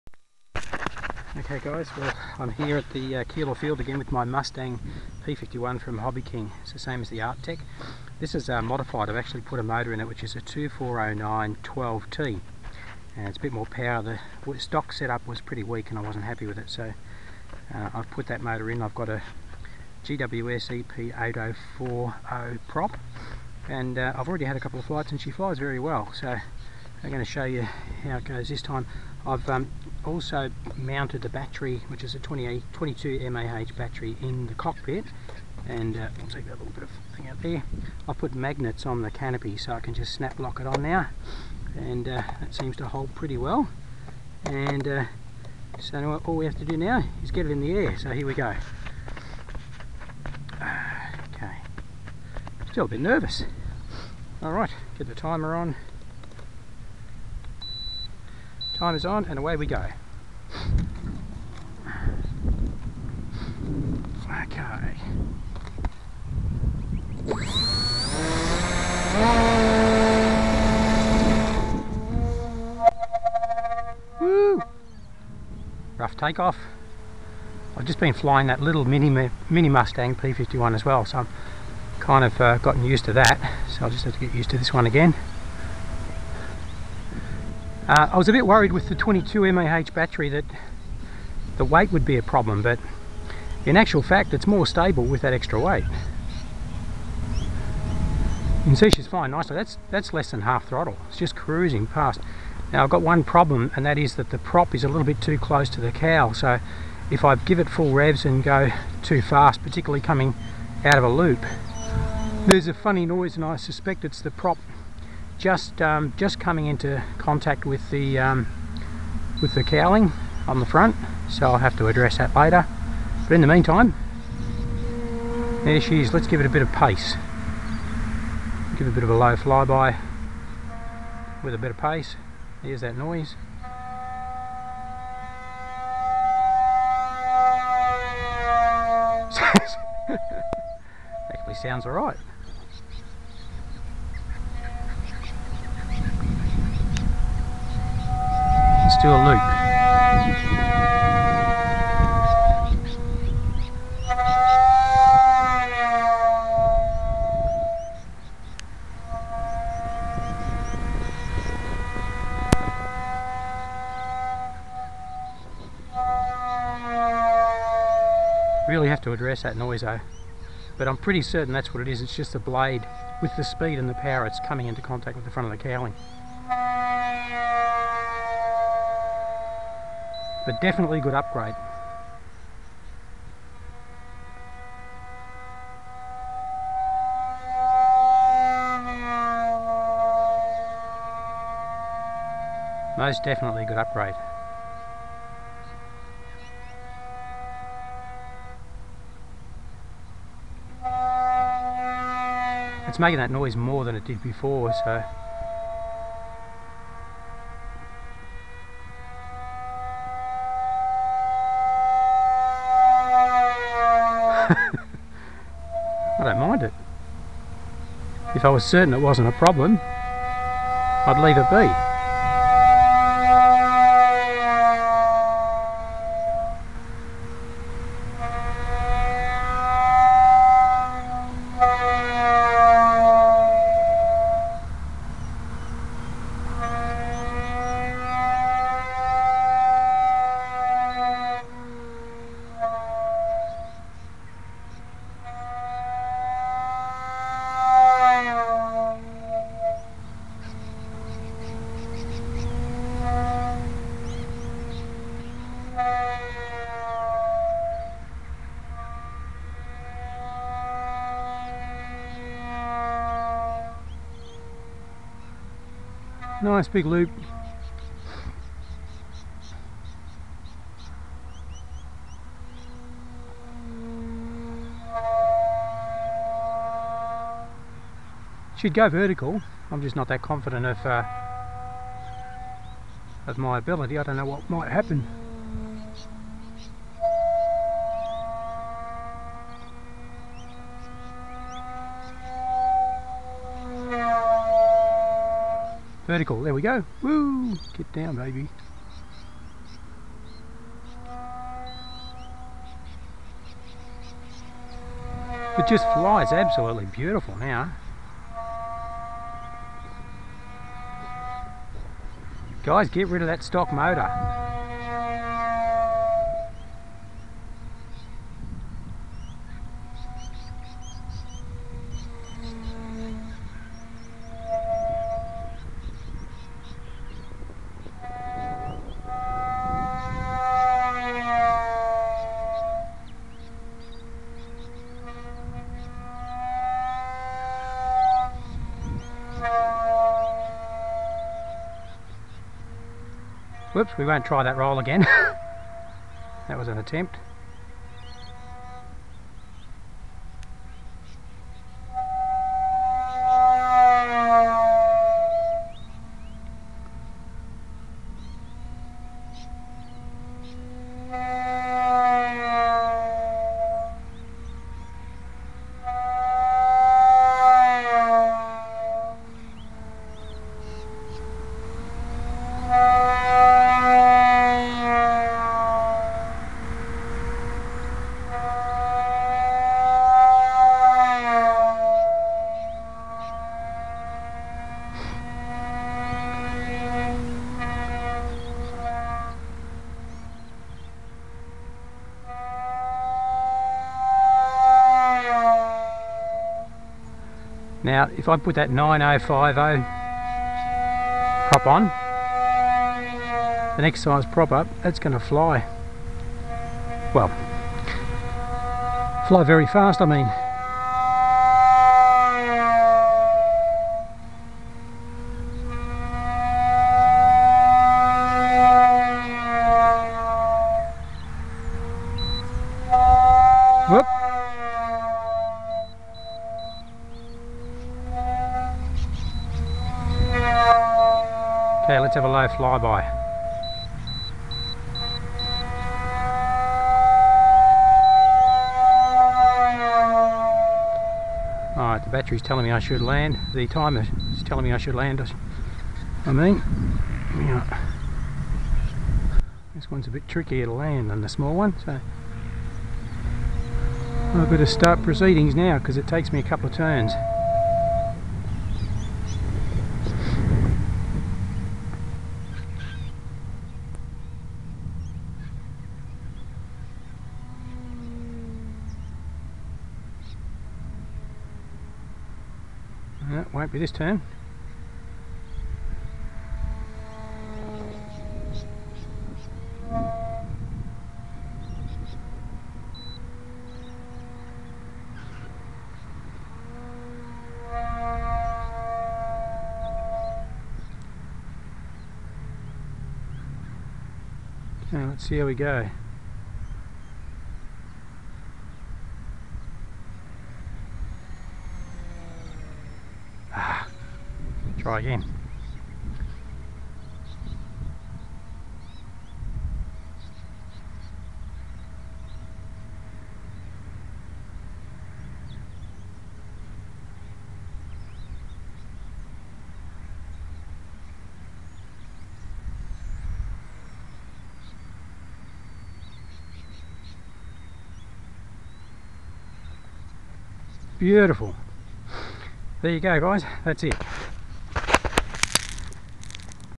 Mustang P-51 (Hobbyking - Art Tech) MODIFIED
2409-12T 1600kv brushless, GWS EP 8040 propeller, Hobbywing 30A ESC, 2200mah Lipo in the cockpit, magnetic removable canopy.
The added weight of the 2200mah battery made this plane very stable in the air.
Love how it flies now. Waiting for a GWS EP 9050 propeller to arrive. That will increase thrust and speed even more.